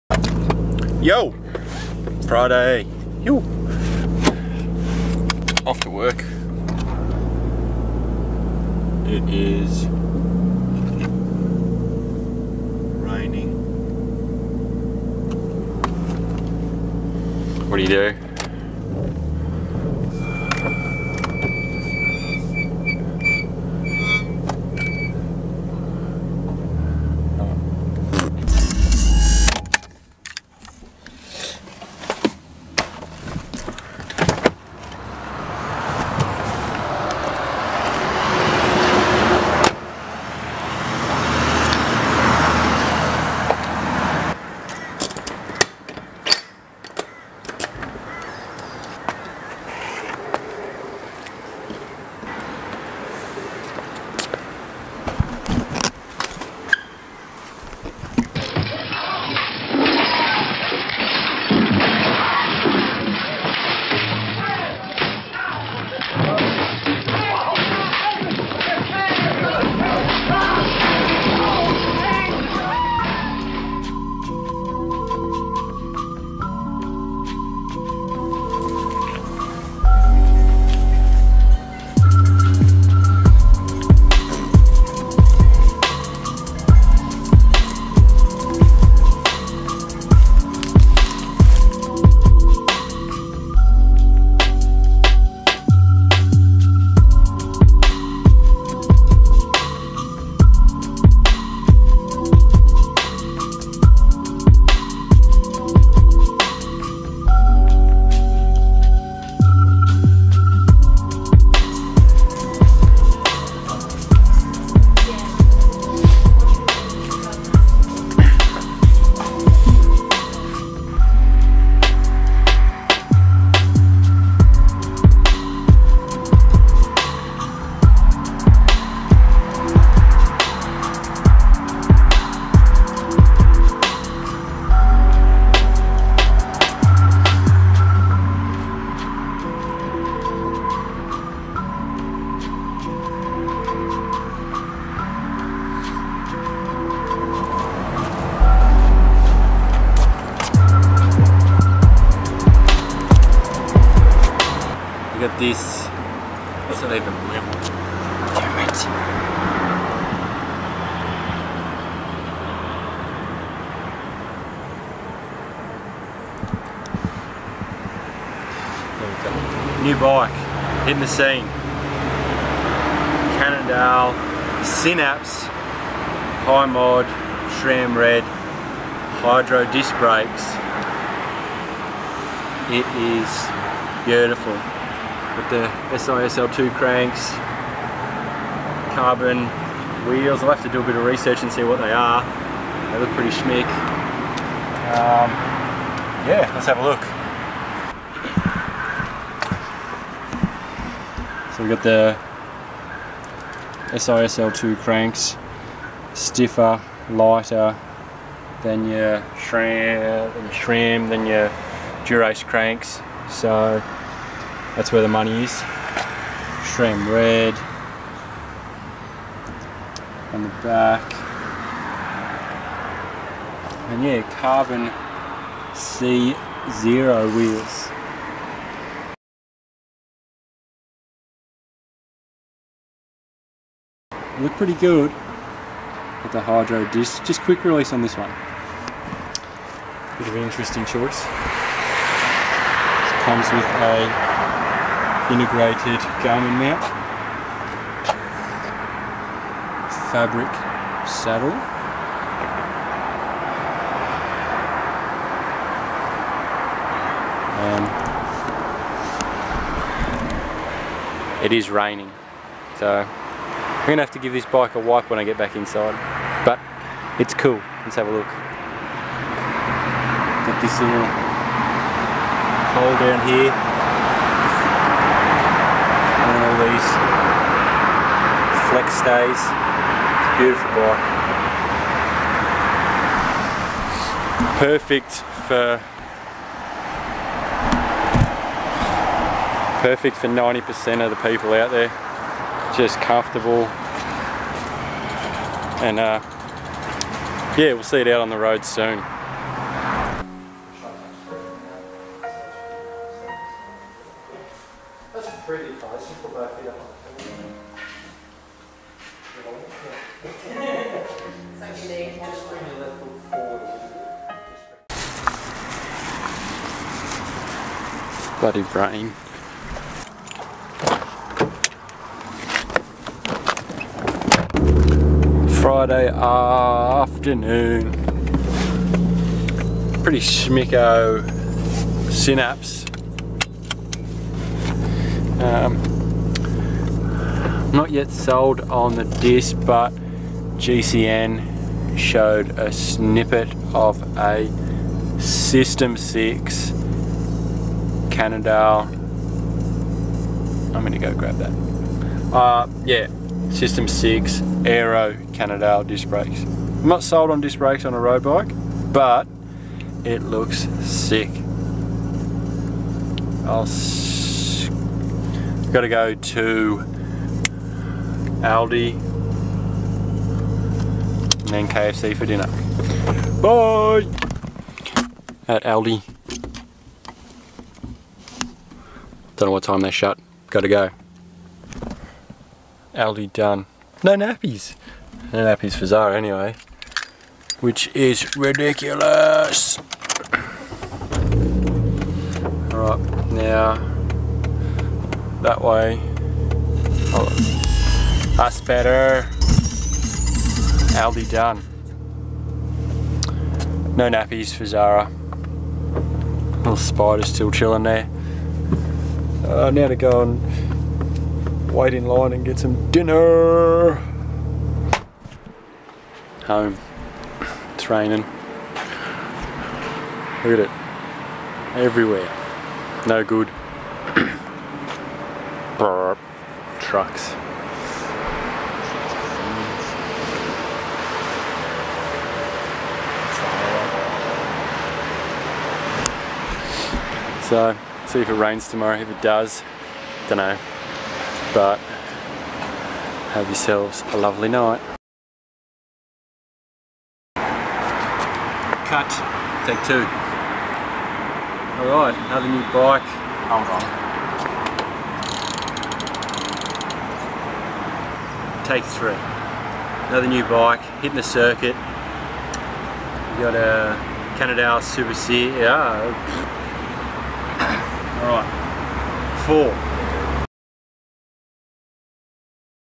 I Built a Cannondale Synapse disc!
Rain

Warpped a Giants bars

Built a Synapse hi mod! red22 disc!